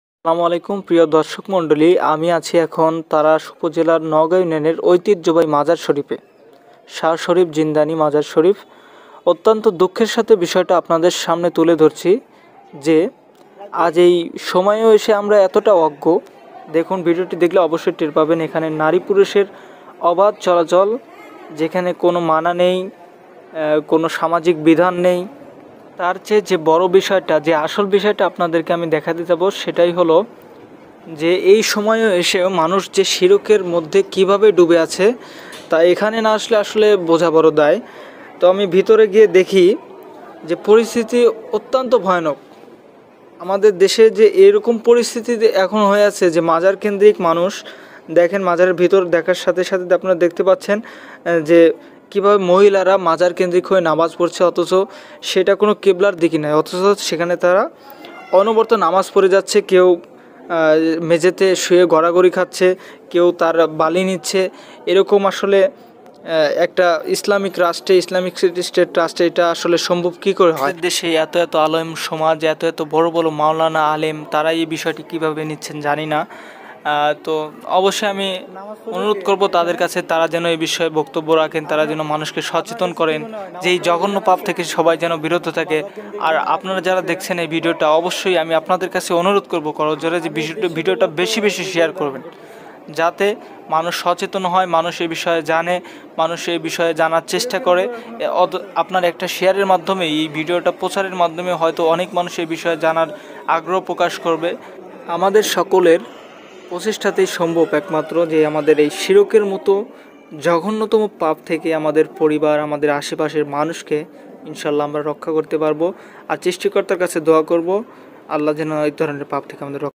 চোখের সামনে কিভবে শিরক করে বেড়াচ্ছে মানুষ
শিরক একটি জঘন্যতম পাপ। মানুষ মাজার পূজারী হয়ে গেছে নামাজ পরছে আমাদের সকলের সামনে দেখার কেউ নেই।ভিডিওটির  মাধ্যমে তুলে ধরার চেষ্টা করেছি নওগাঁ ইউনিয়ন মাজার শরীফের ভিতরের কিছু চিত্র। আমি আশা করি  অনেক সতর্ক হবেন। যারা অলেম ওলামা অাছেন অবশ্যই বিষয়ে আরো বেশি বেশি সতর্ক করবেন এদেশের মানুষকে।